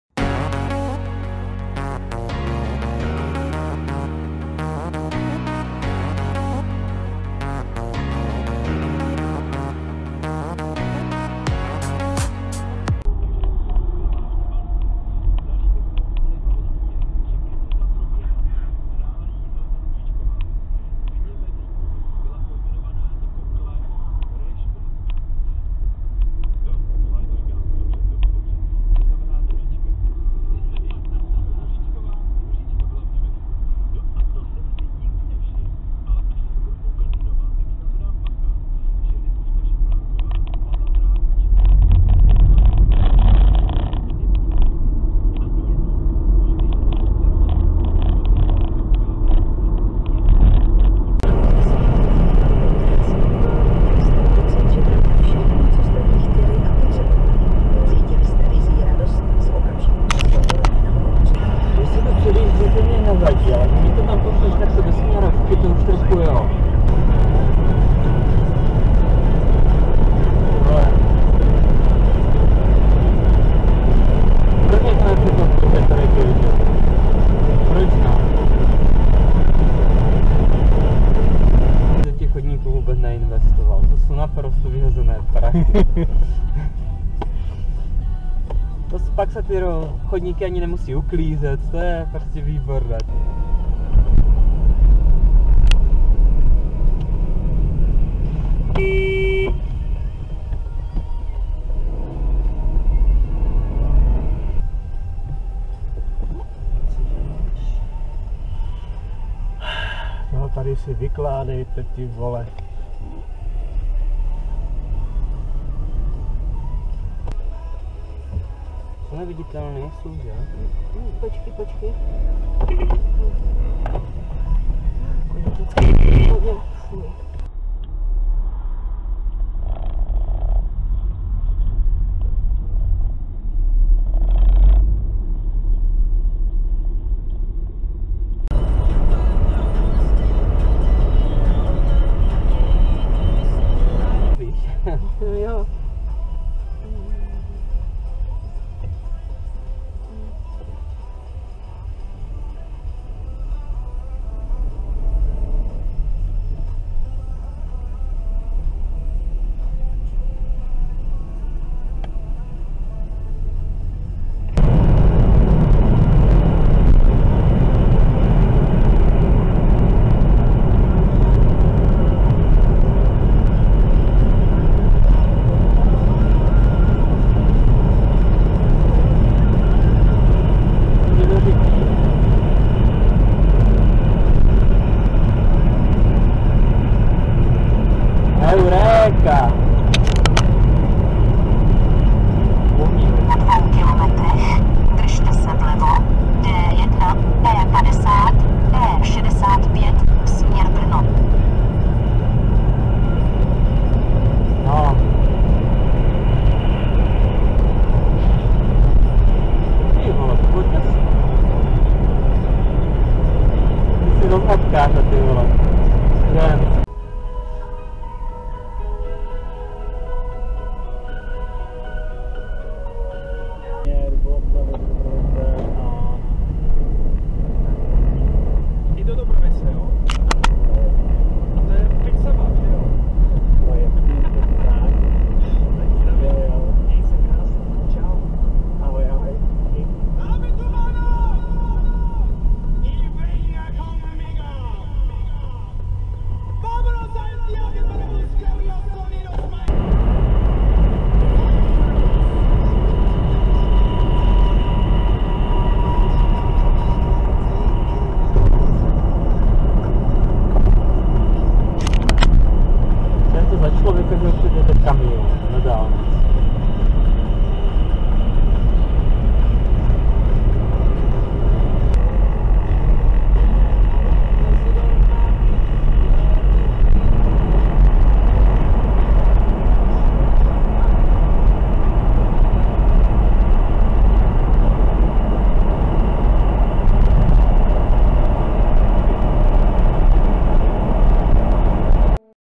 Málem jsem srazil děcka na koloběžce!, Paní levačka, Úprk, Kamiony a Dálnice - Videa ze silnic č. 62
Dnes uvidíte:
0:00 intro
0:13 Nehoda a zipování
0:52 Vtíravý kamion
1:07 Regiojet v prostředním pruhu
1:22 Chodník
1:33 Nedání přednosti
1:45 Povídání v křižovatce
1:58 Děti na koloběžce
2:07 Chodci na červenou
2:19 Porucha
2:25 Matení nepřátel
2:45 Paní levačka
3:37 Úprk
3:44 VIP pruh
4:10 Kamion předjíždí osobák
4:32 Efektivita využití pruhů
4:52 Outro